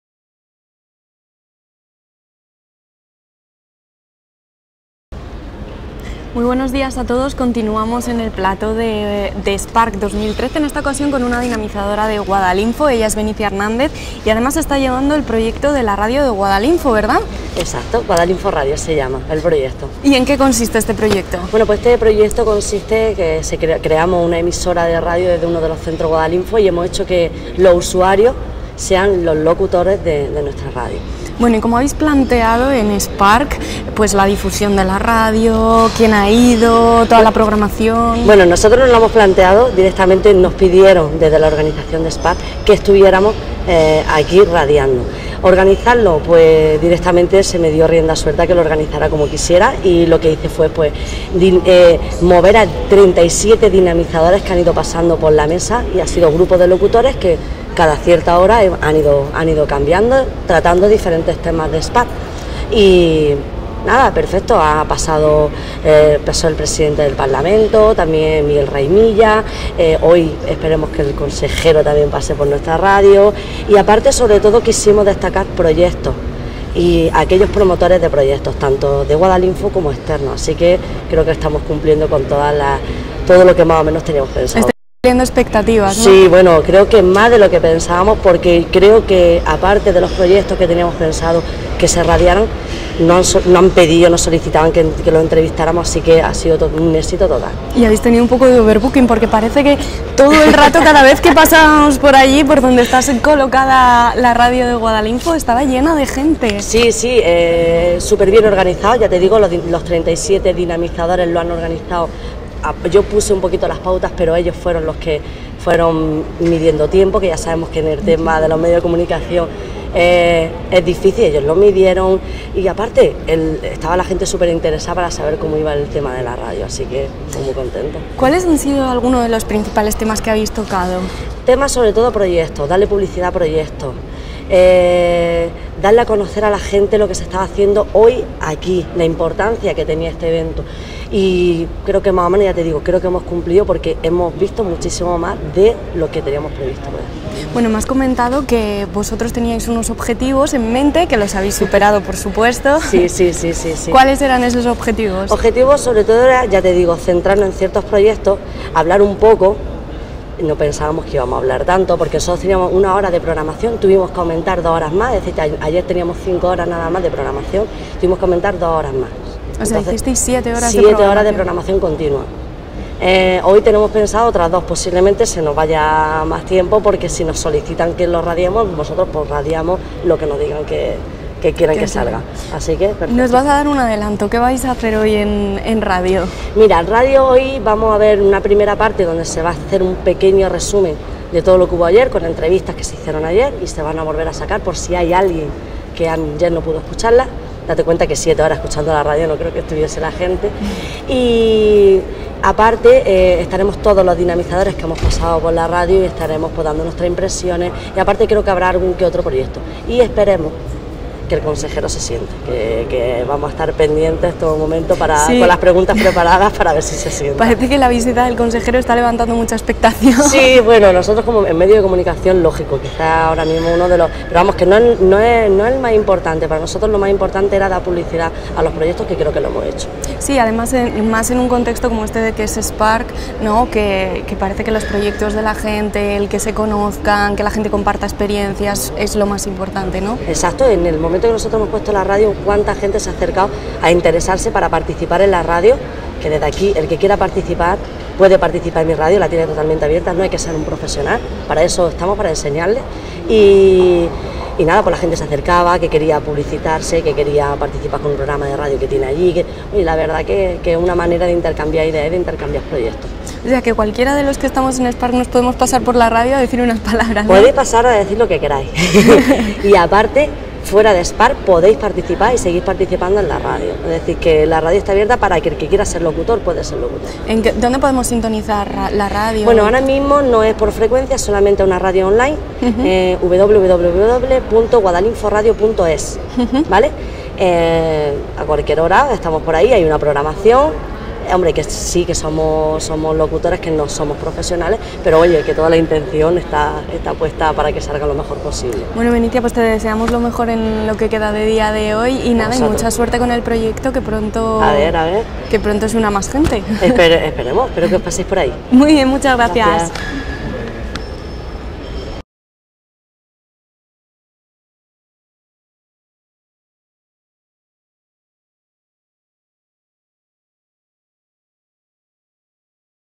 Spark13: Benicia Hernández (Guadalinfo Radio)
Entrevista en Spark13 con Benicia Hernández, una de las responsables del proyecto Guadalinfo Radio, que cubrió en directo el IV Foro Global de Telecentros.